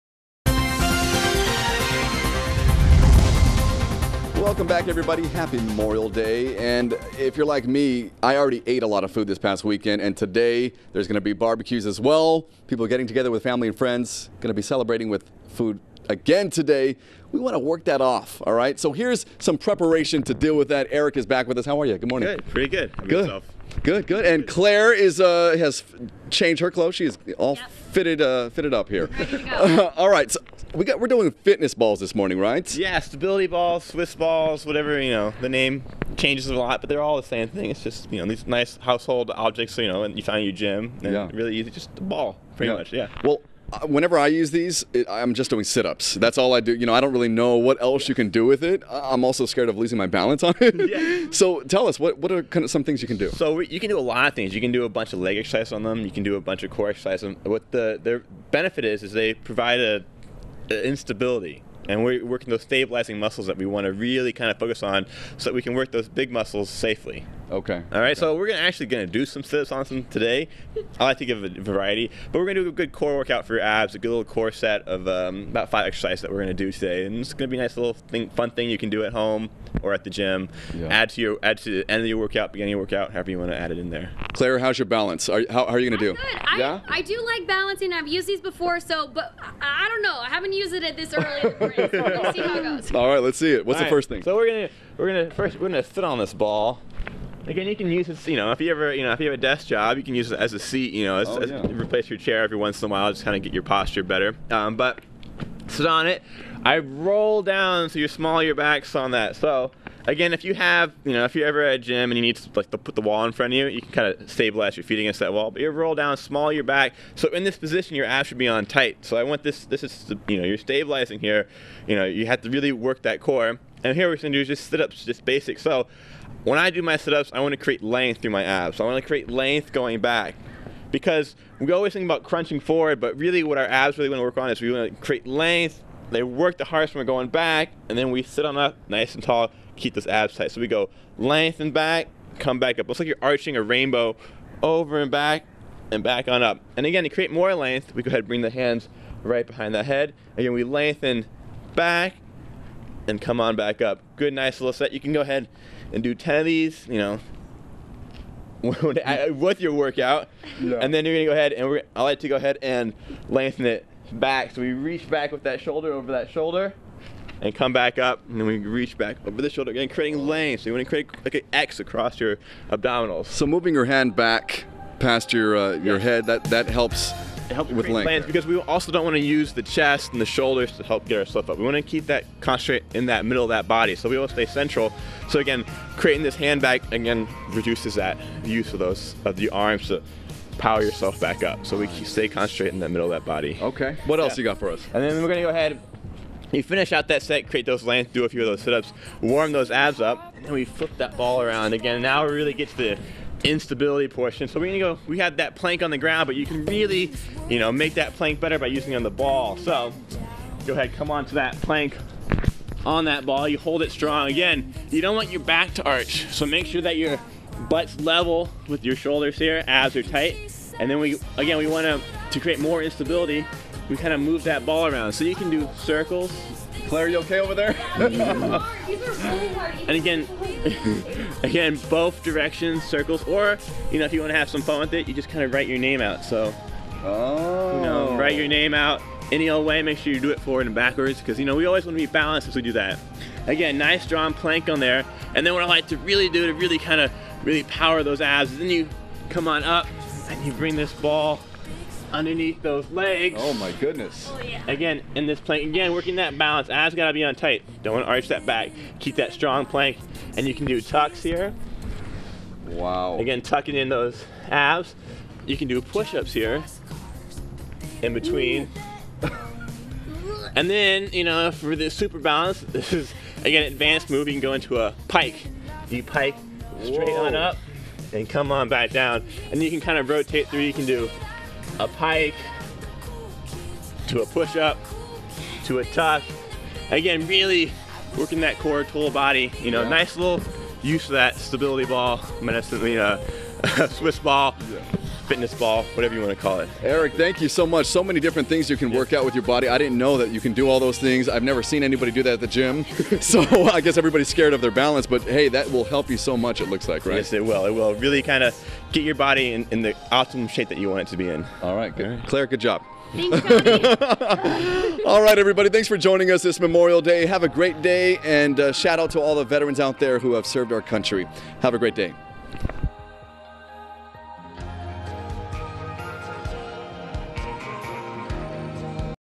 KKFX Morning: How to Work Out with a Fitness Ball
Eric Mundstock of Boot Camp SB, shows us how to get in shape with a fitness ball.